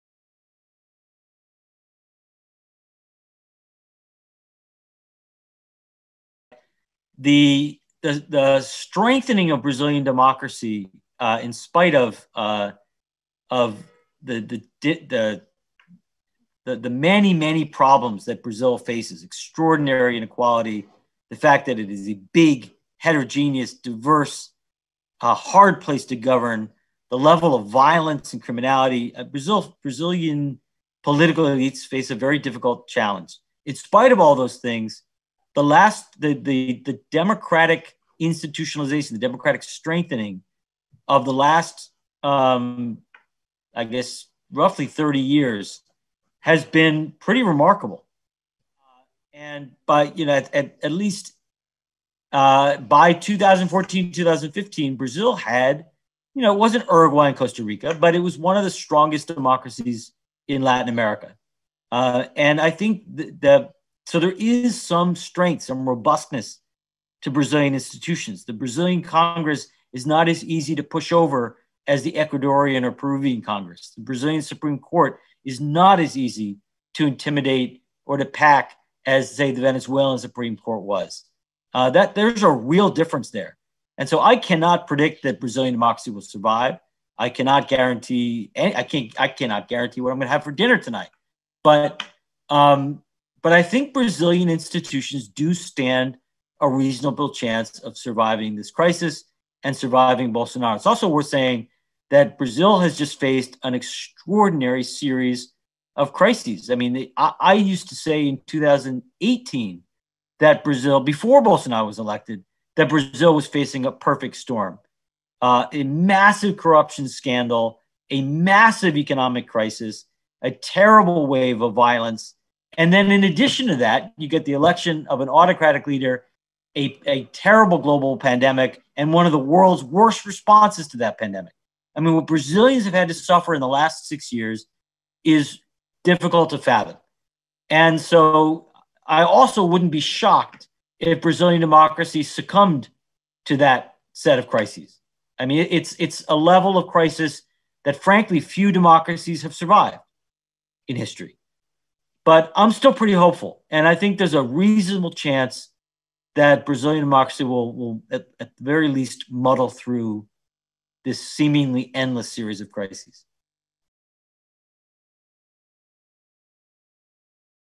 Steven Levitsky: "A Suprema Corte Brasileira não é tão fácil de intimidar como no caso da Venezuela"
Autor do livro "Como As Democracias Morrem" é o primeiro convidado da série que debate os desafios da democracia brasileira. Veja a íntegra do webinar da Casa JOTA com Steven Levitsky